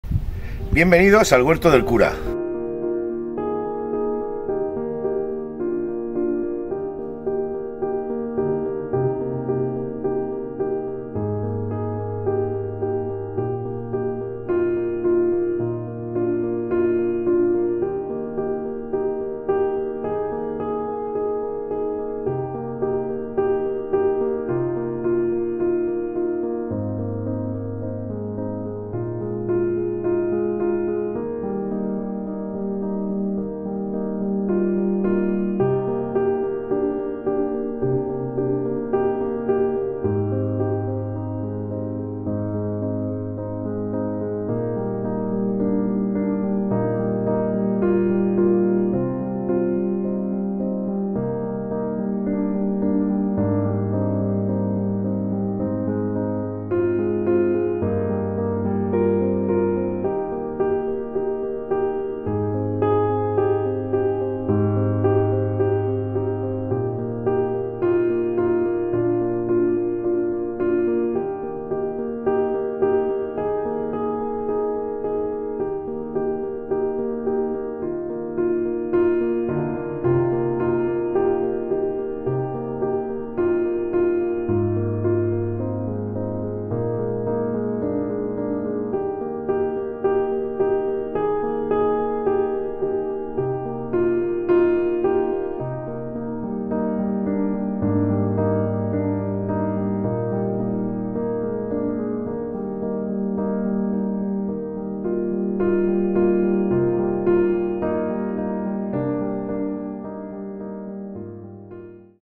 Bienvenidos al Huerto del Cura
Bienvenidos a este video donde se muestra un pequeño corto de nuestro huerto, espero que os guste familia!